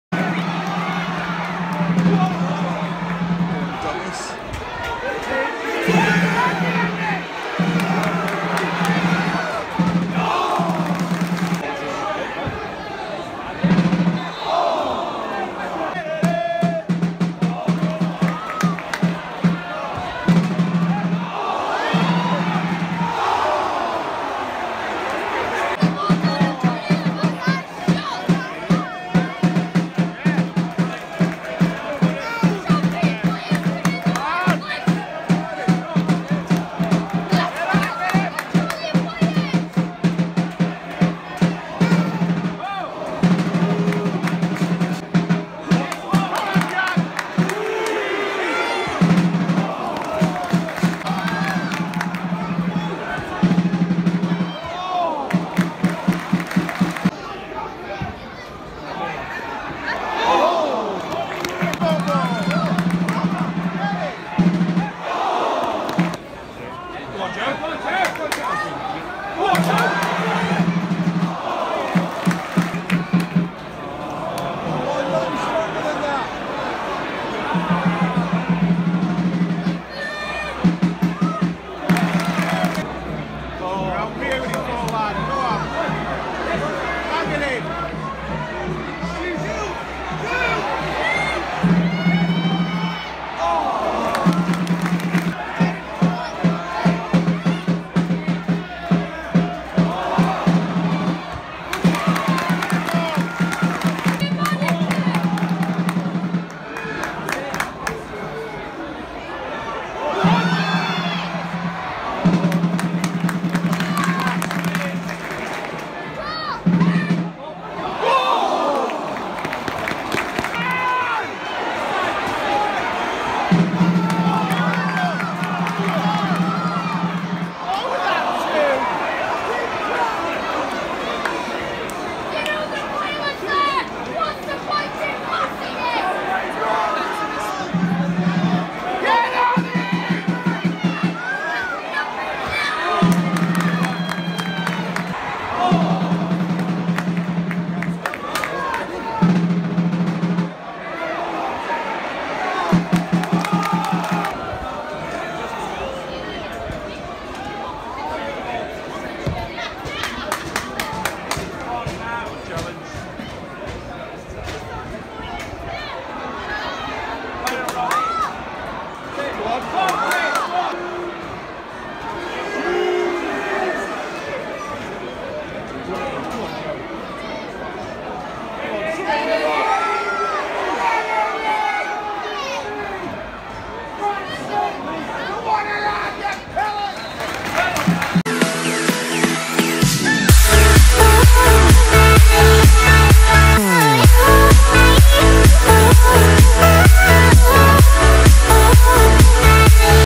Chestefield v Colchester
these are the match highlights from Chesterfield v Colchester in EFL league two and i hope you enjoy it